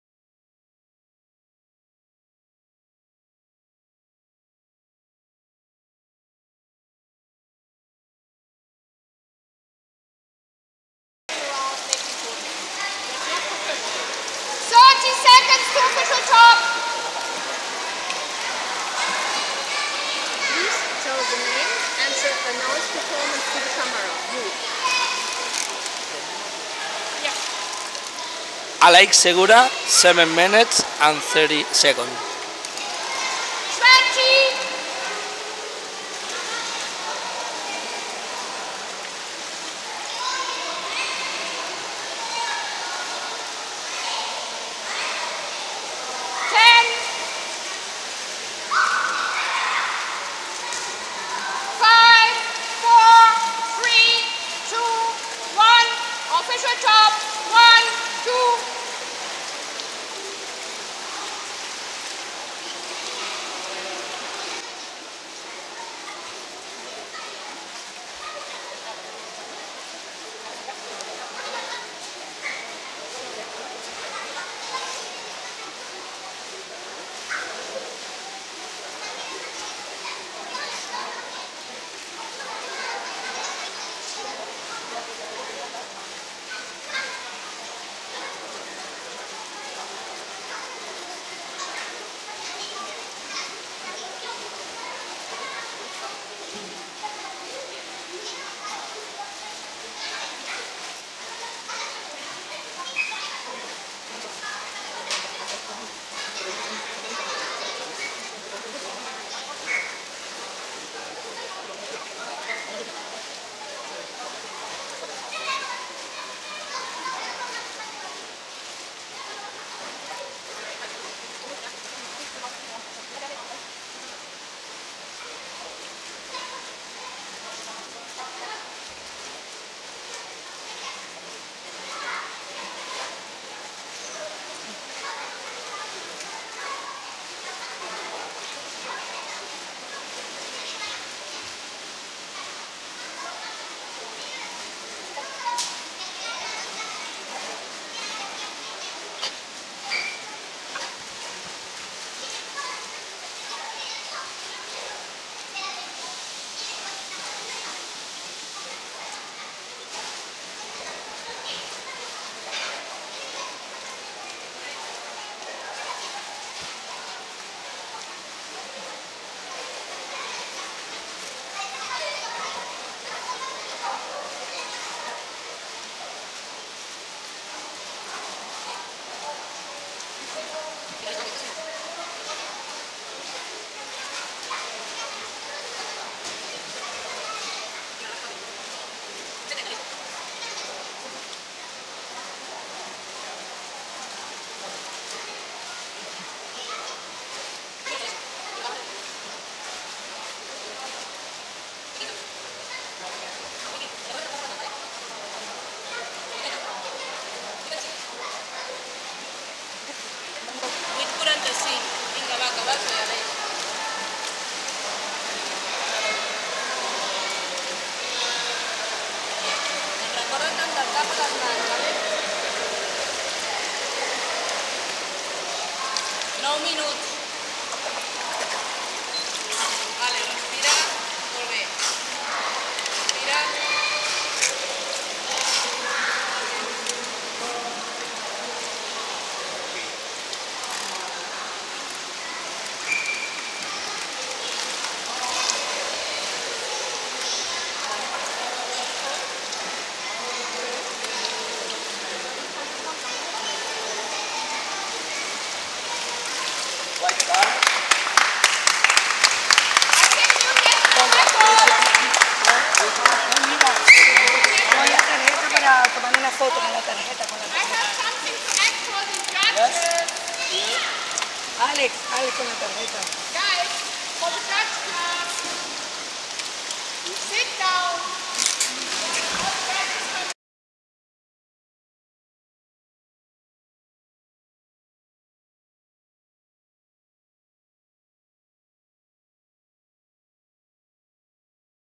Record STA AIDA España Aleix Segura
El 20 de abril en Palafrugell se realizó la 1ª prueba de apnea 2014 de AIDA España.
Aleix Segura consiguió record de apnea estática con 09'04''